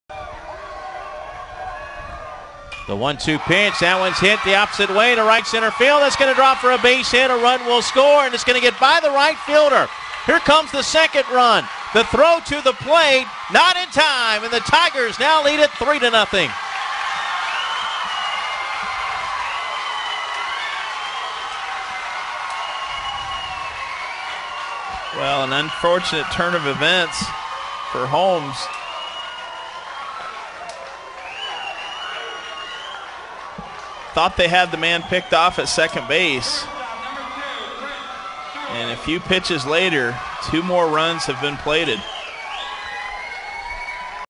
#5 Kyle Shealy of Union County with a 2-run RBI double at 2012 FHSAA 1A Baseball Final
#5 Kyle Shealy of Union County with a 2-run RBI double at 2012 FHSAA 1A Baseball Final at Domain Park in Port St. Lucie, Florida